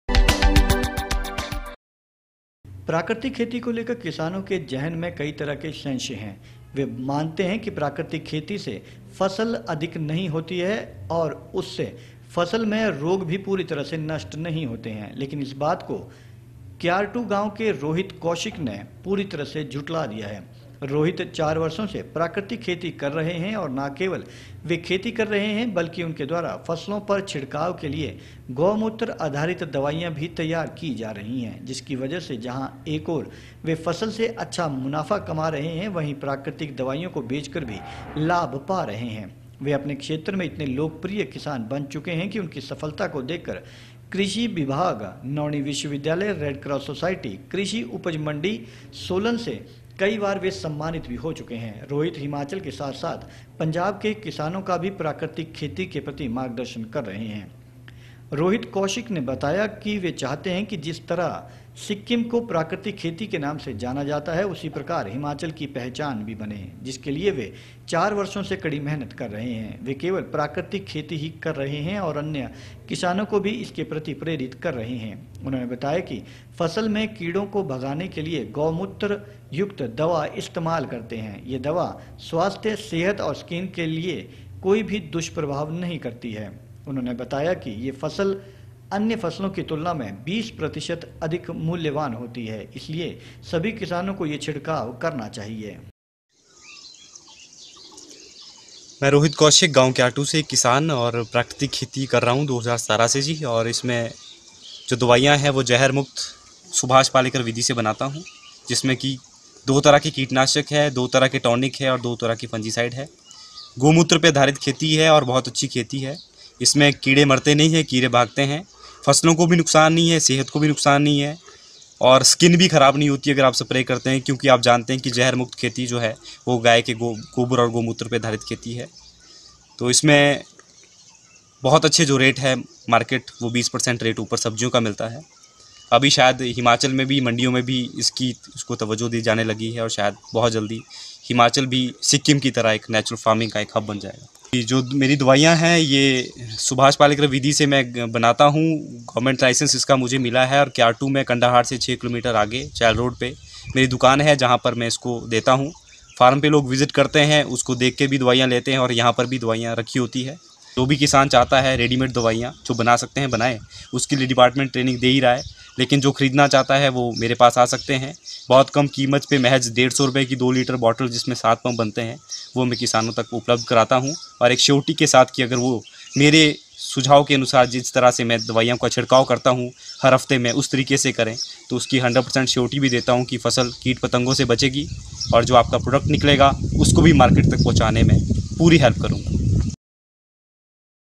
SOLAN क्यारटू गाँव के किसान रोहित ने प्राकृतिक खेती में पंजाब तक बनाई पहचान
क्यारटू गाँव के किसान रोहित  ने प्राकृतिक खेती में पंजाब तक बनाई पहचान